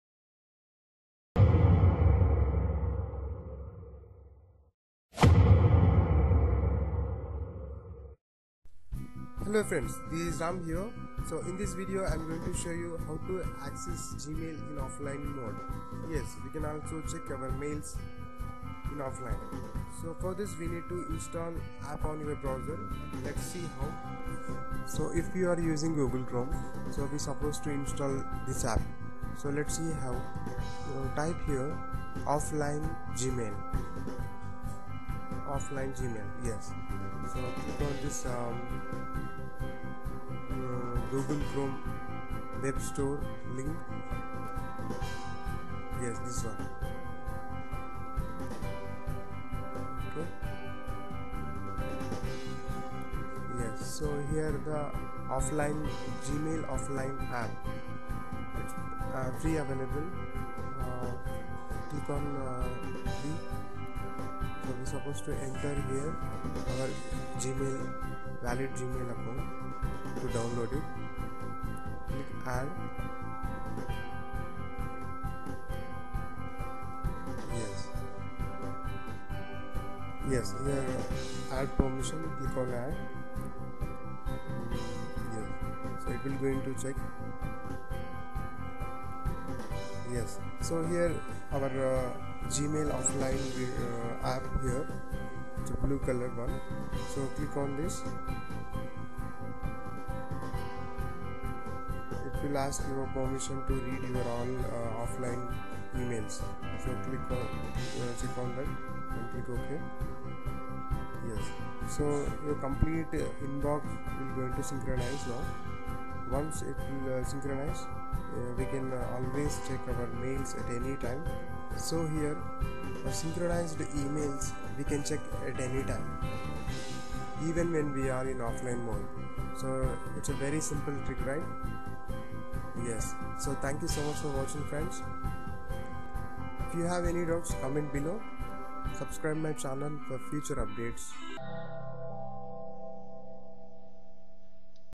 How to Access Gmail in Offline Mode Without Internet For Free
in this video i showed you "How to Access your Gmail without Internet,Once we install the App from Google Chrome Web store than we will access in offline Mode.